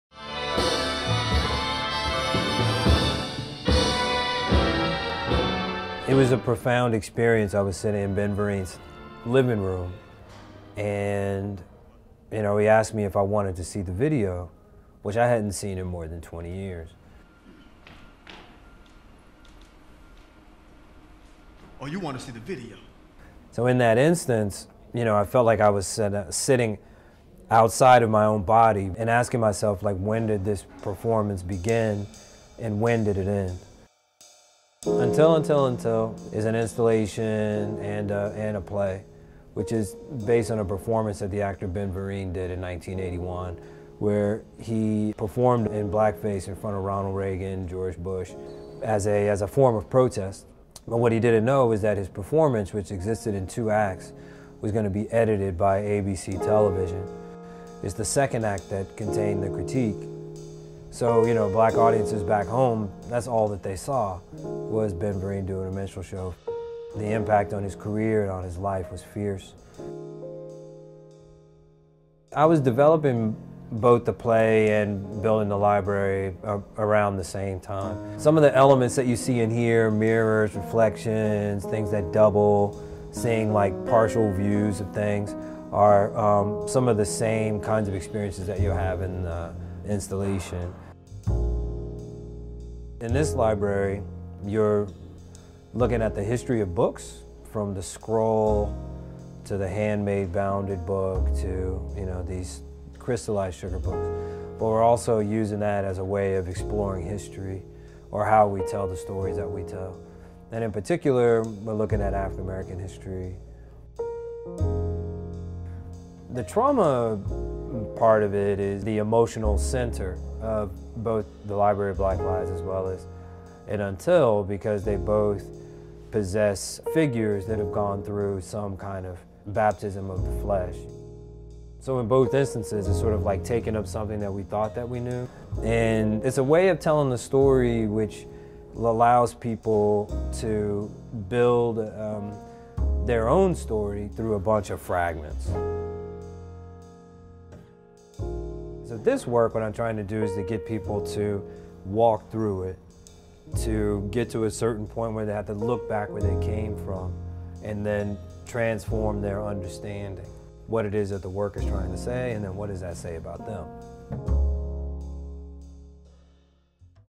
EDGAR ARCENEAUX | Until, Until, Until… | Library of Black Lies | YBCA
Los Angeles–based artist and YBCA 100 honoree Edgar Arceneaux explores historical patterns around race and representation through drawings, installations, and theater.

At YBCA, the artist experiments with the exhibition as a format of presentation. Until, Until, Until… (2015–17) is a multi-media video installation as well as a live action play—written, staged, and directed by Arceneaux—that revisits a controversial performance by Ben Vereen at Ronald Reagan’s inauguration. Complementing this work, Library of Black Lies (2016) is a large-scale sculpture and labyrinth containing a collection of crystallized books—from dictionaries, to encyclopedias, to novels—presenting variants on African American history.

Until, Until, Until… will be performed live in the gallery space on February 22–24, 2018.